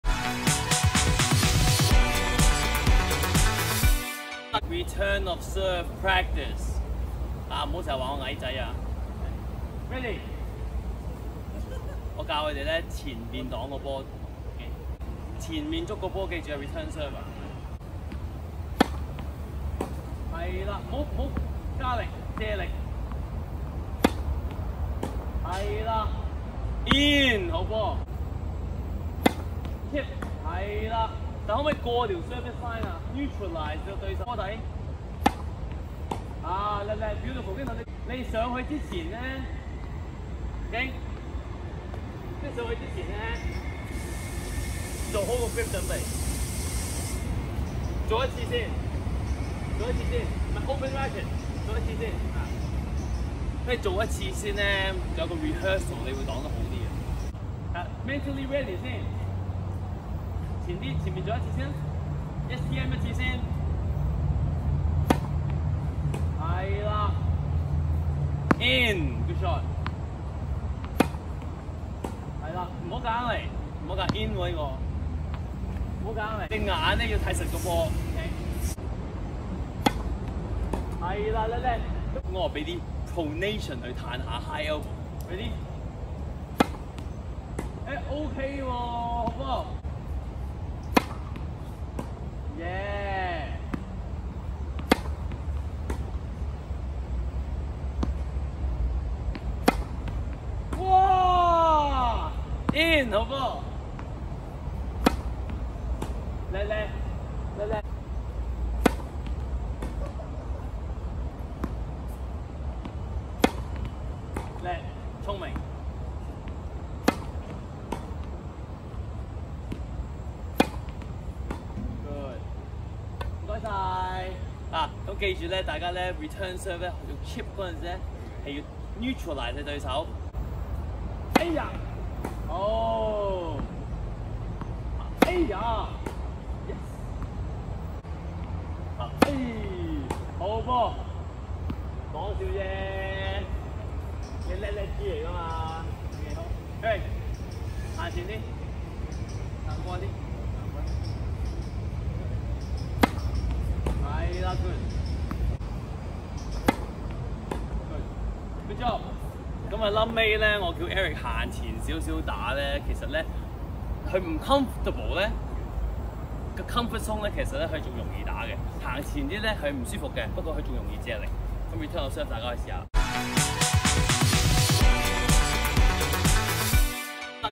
(廣東話) Henry So蘇浩剛國際網球導師及教練 “CHIP” RETURN OF SERVE 訓練🎾😮🔥 Henry So蘇浩剛國際導師及教練
Henry So蘇浩剛國際網球導師及教練 “CHIP” RETURN OF SERVE 訓練🎾😮🔥

如有興趣請 msg 61357606